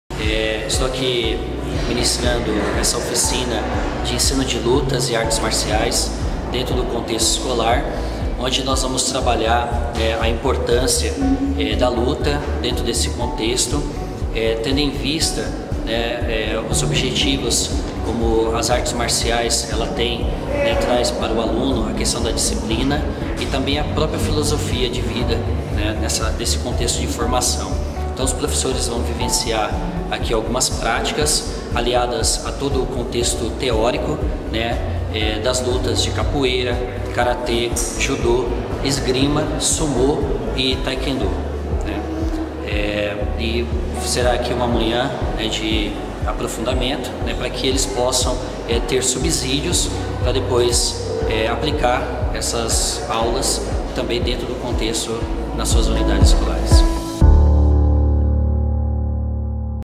Oficina de lutas e artes marciais marca III Orientação Técnica
O Professor Fabrício Augusto Ribeiro ministrou a oficina "Ensino de lutas, artes marciais e modalidade de combate no contexto escolar", para os professores de Educação Física da Rede Municipal de Ensino de Assis no último dia da III Orientação Técnica - Replanejamento: Educação em Movimento da Secretaria Municipal de Educação - SME.